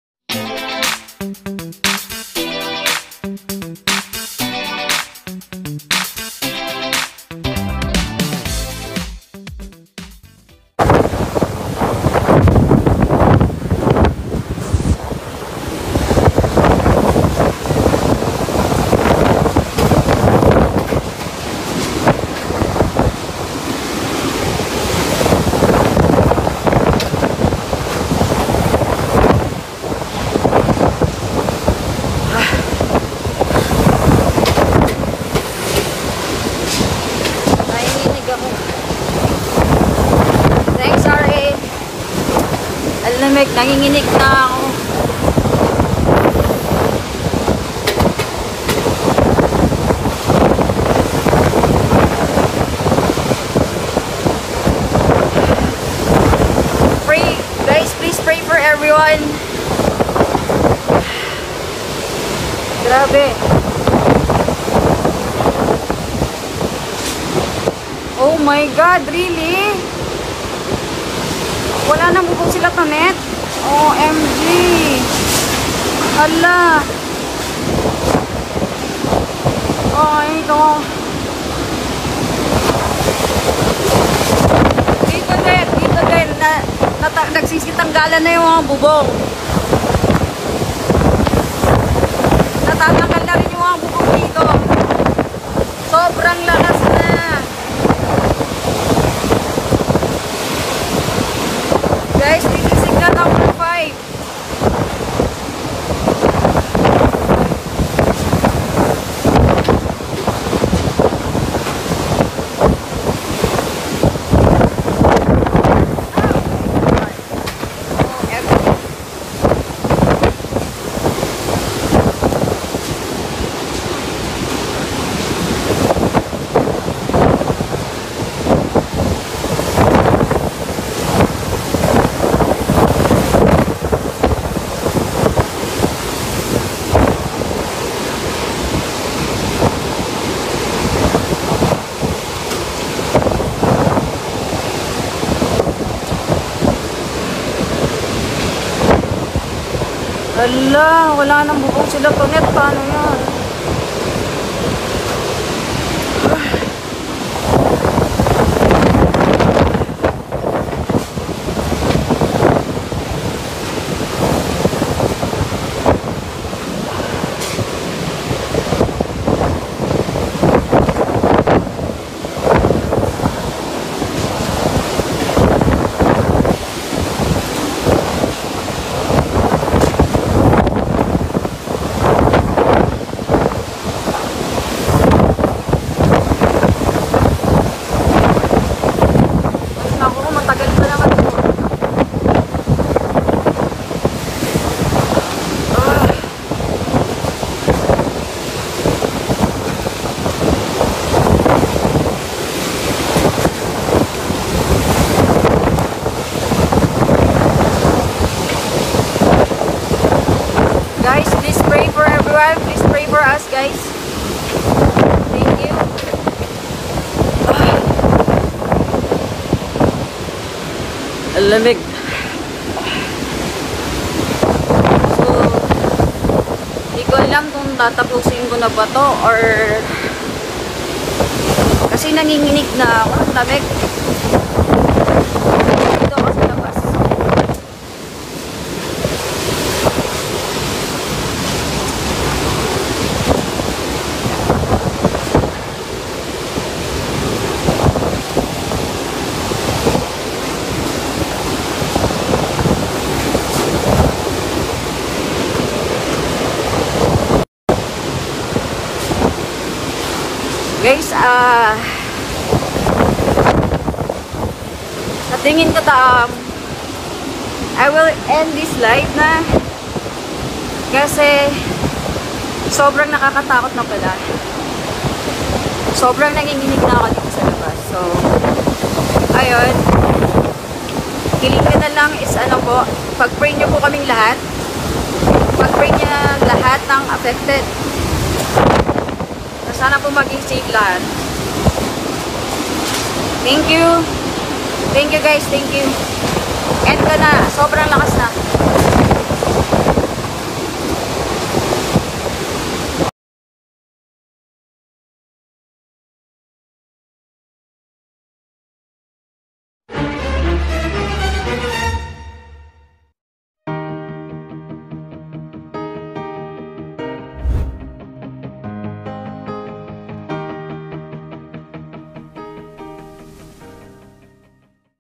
Typhoon Rolly Rumagasa Na Po Sa Camsur Bicol | AmoreVlogs
❤️mag ingat po tayo..

❤️ AmoreVlogs